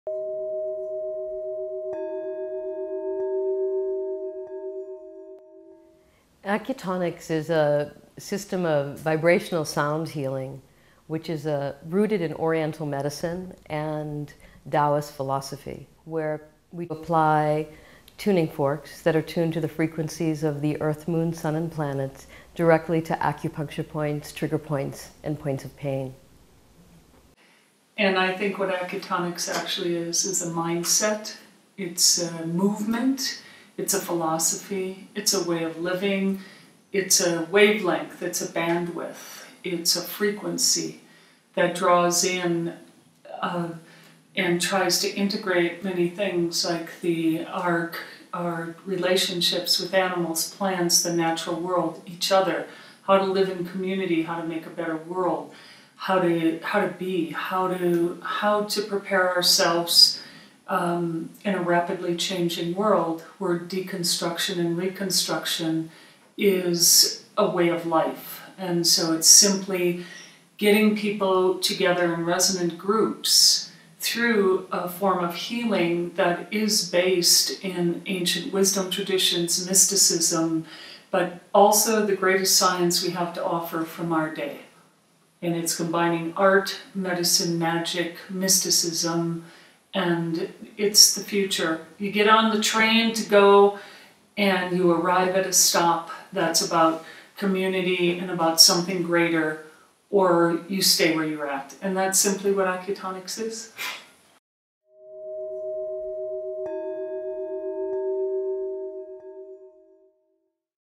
Acutonics Integrative Medicine
This video provides a brief description of Acutonics from co-founders, Donna Carey, LAc and Ellen F. Franklin, PhD. Vibrational sound healing, rooted in Oriental medicine, Taoist philosophy, and sound. A movement, philosophy, and way of living in the 21st Century, that brings people together in resonant groups, to study, practice, do important work for the healing of individuals, animals, and the planet. Acutonics is rooted in science, ancient wisdom traditions, psychology, sound, Oriental medicine and Taoist philosophy.